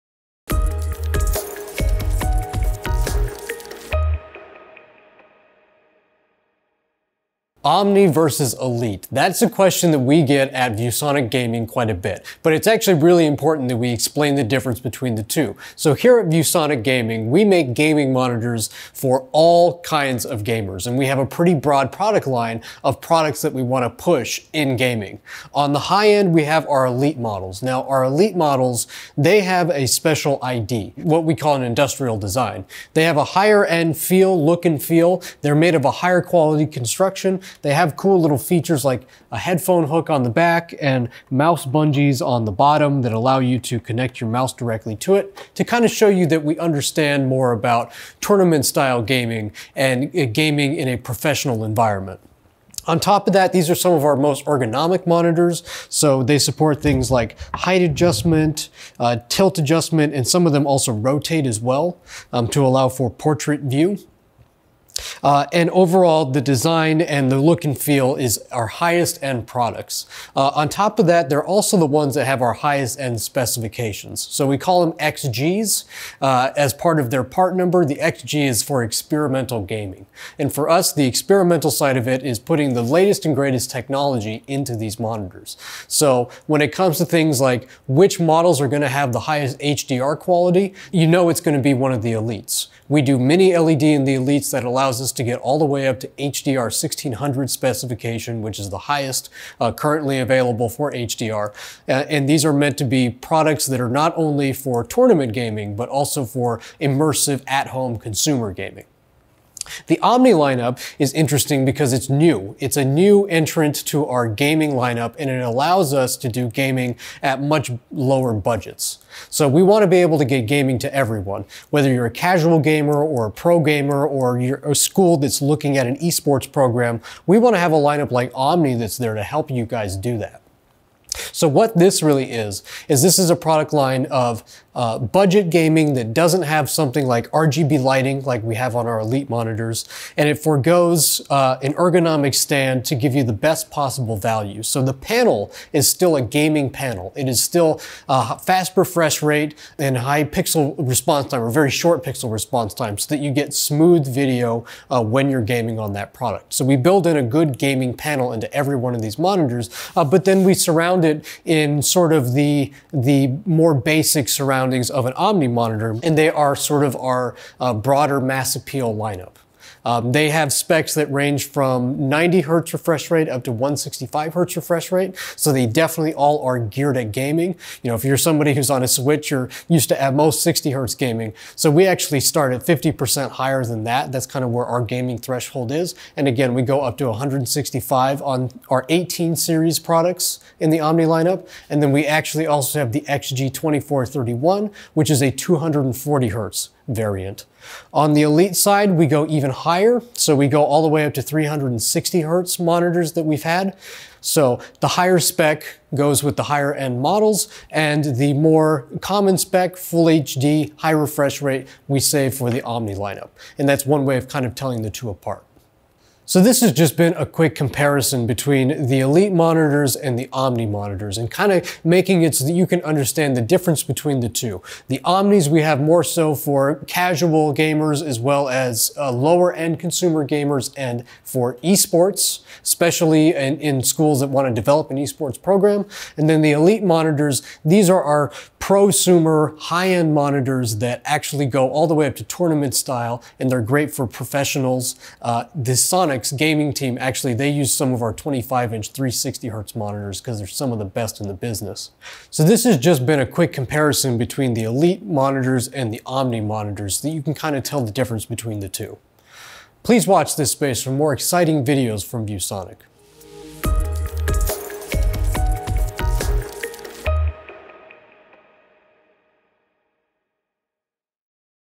ViewSonic Gaming Monitors: OMNI vs. ELITE
ViewSonic gaming monitors are engineered to deliver best-in-class features for all types of gamers. The ViewSonic OMNI™ line of monitors are built for everyday gaming, while the ViewSonic ELITE™ line of monitors deliver professional-grade features for esports and competition.